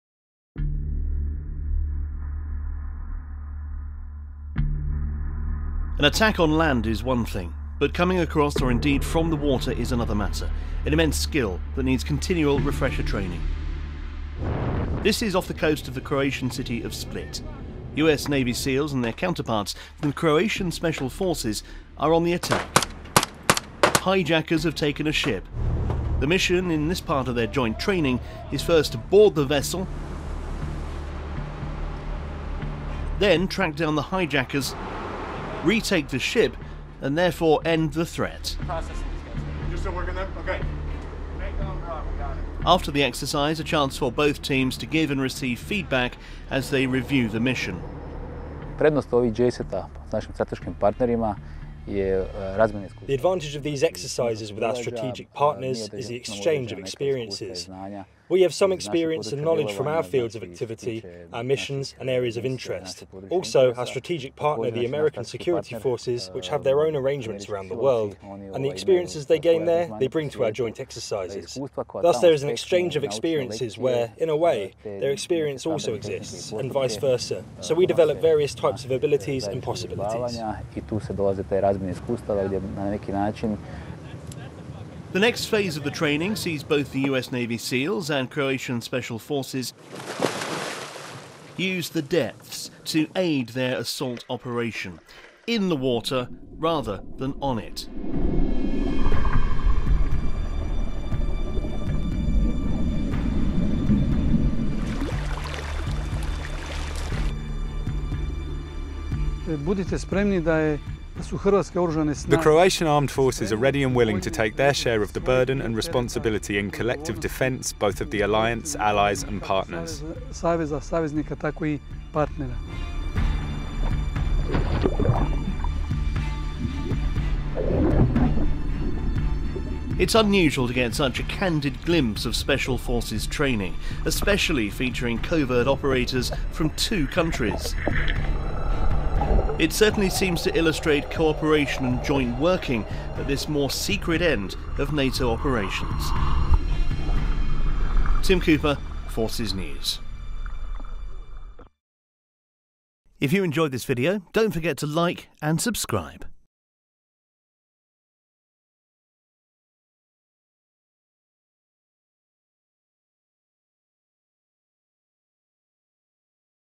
Rare glimpse of US Navy Seals and Croatian Special Forces on joint exercise
Croatian special forces have teamed up with the US Navy Seals to hone their water operating skills.

They've been training in the Adriatic Sea.

They offered a candid glimpse of the NATO Special Operations Forces training.